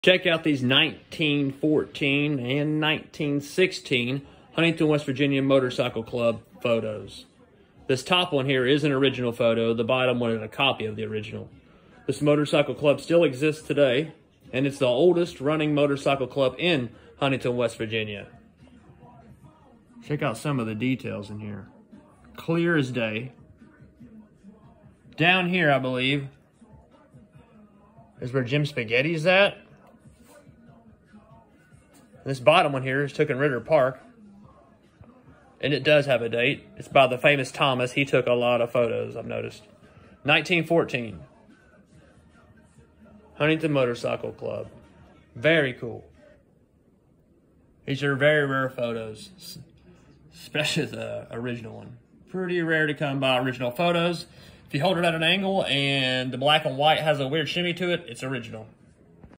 Original 1914 black-and-white photos of the Huntington, West Virginia motorcycle club HMC.
1914 Original photos of the Huntington motorcycle club out of Huntington, West Virginia. They are still running strong today and they are the oldest motorcycle club in the state of West Virginia.
NFS.